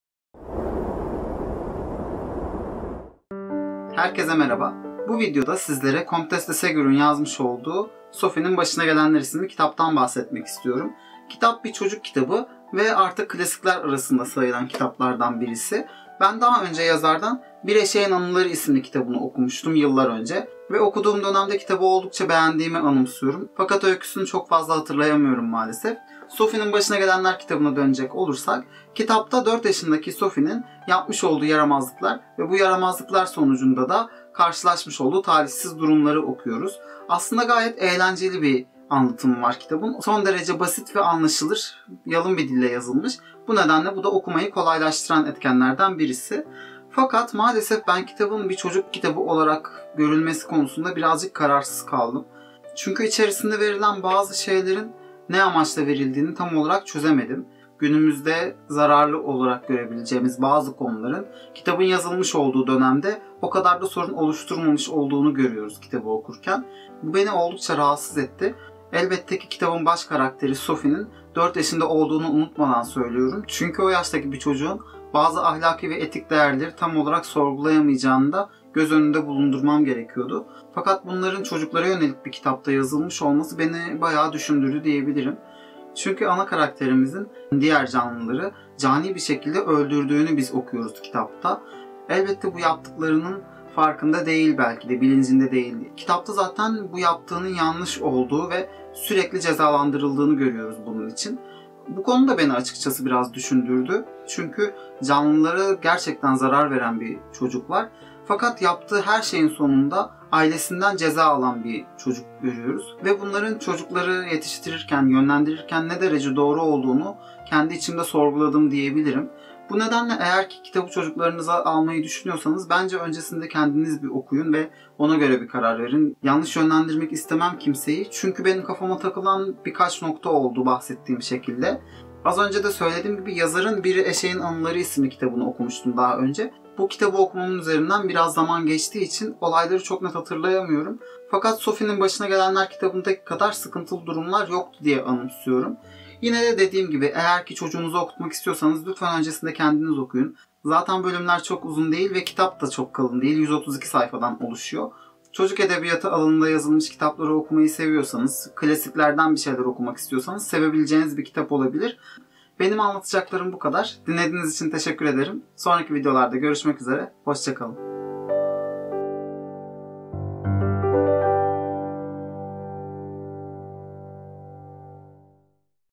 Sophie'nin Başına Gelenler - Comtesse De Ségur | Kitap İncelemesi #60
Herkese merhaba! Altmışıncı kitap incelemesi videomla karşınızdayım! Bu hafta Comtesse De Ségur'un Sophie'nin Başına Gelenler isimli kitabı hakkındaki düşüncelerimi paylaşıyorum. Bazı hatalarım olabilir ama umarım beğenirsiniz. Yorumlarınızı bekliyorum. ;)